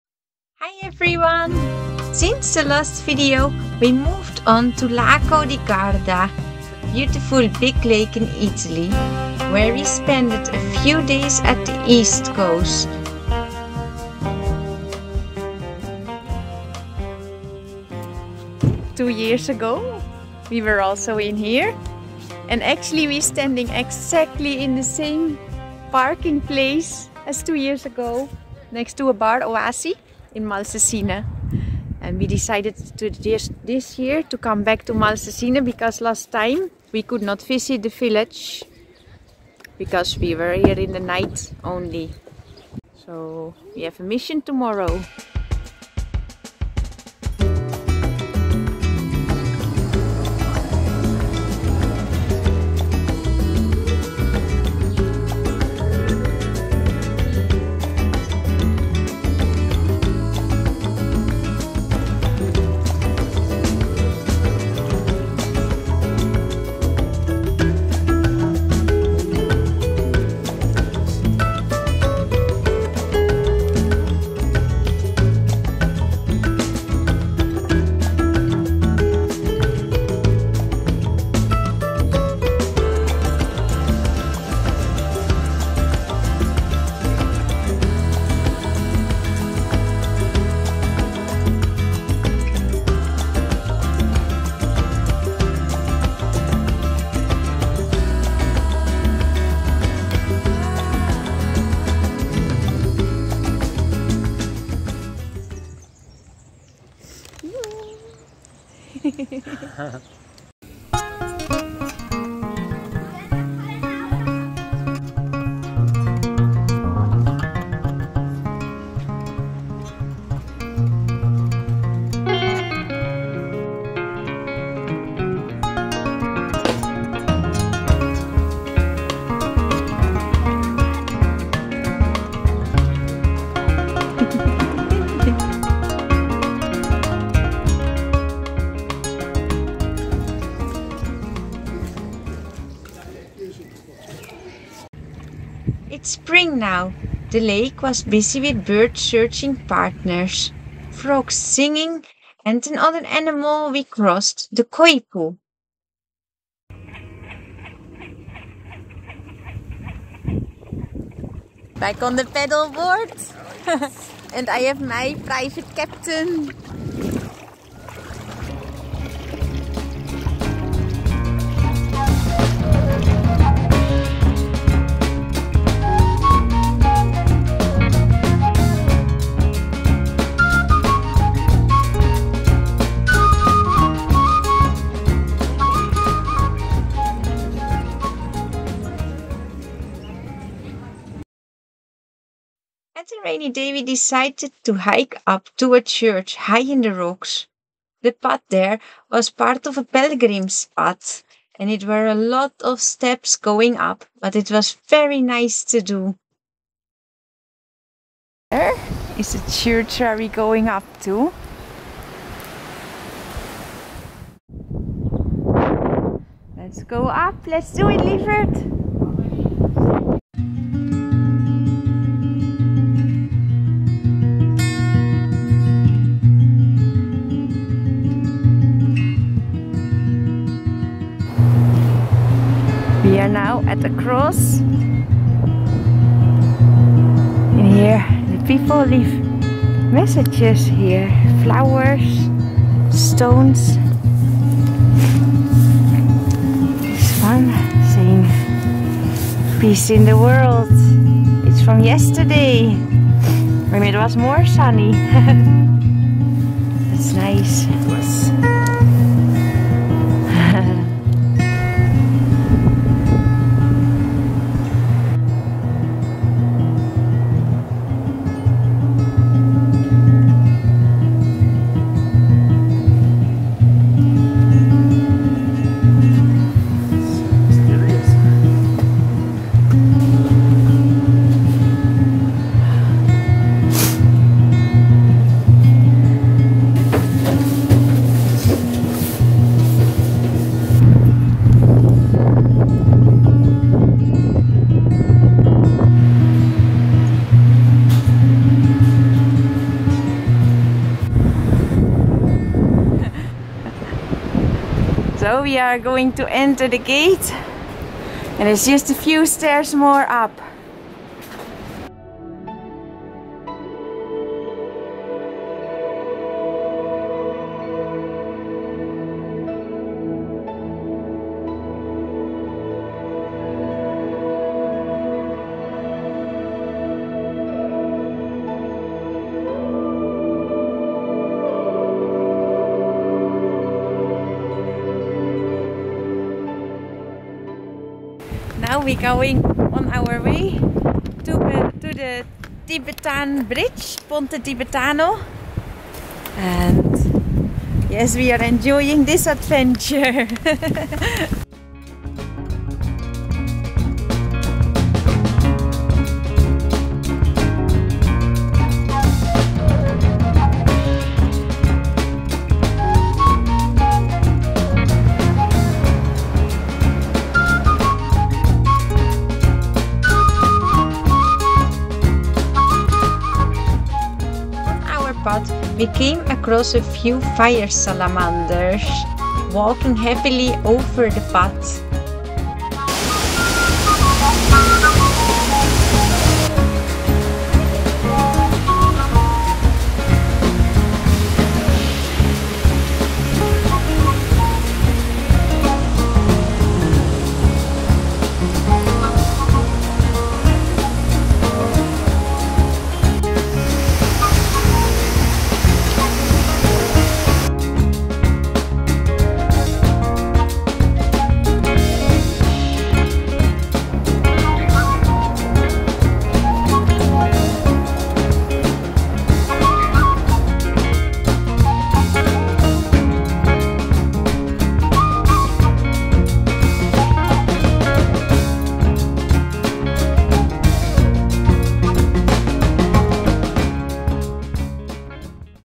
Spring at Lago di Garda
Hello there!

It has been a while since our last video, that's because we did not encounter any wifi on our travel. Now finally I could upload a new video from our travel, althrough this was in April. We went a few days on our trip through Italy to Lago di Garda. Enjoy the video!!

XxX,
Daphne and Ivan